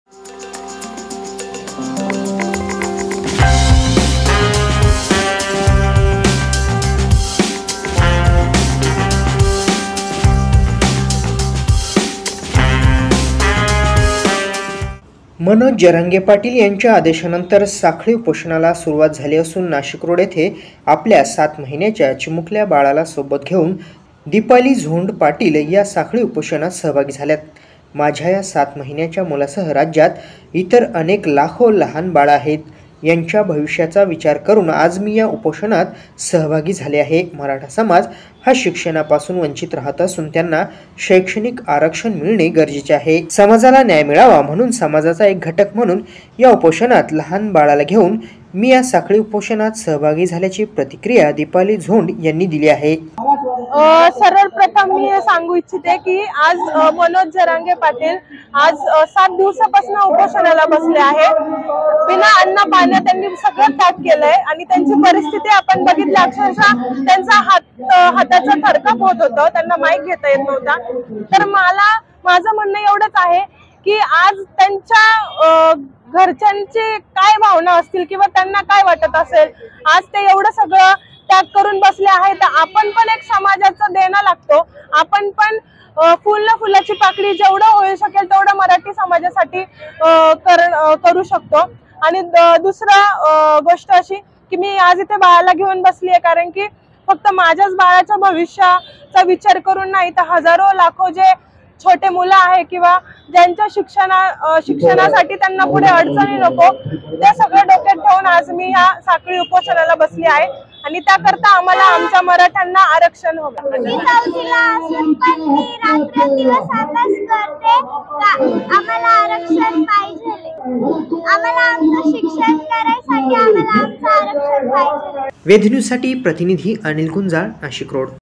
सात महिन्यांच्या लहान बाळास घेऊन माता साखळी उपोषणात सहभागी
Editor-Director: Santosh Kamod, Suhata Yogesh Kamod